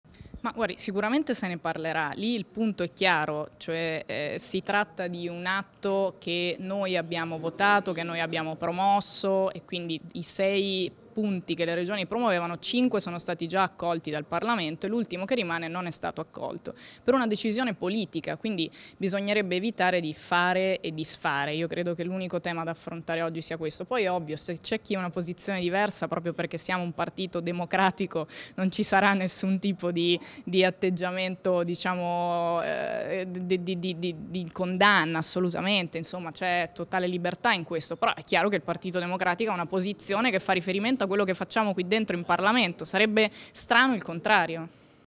ASCANI (PD): AL REFERENDUM TRIVELLE LASCIAMO MASSIMA LIBERTA' PROPRIO PERCHE' SIAMO DEMOCRATICI
(Agenzia VISTA) - Roma, 4 Aprile 2016 - VI festa della Scienza e della Filosofiaq, la conferenza stampa alla Camera dei Deputati.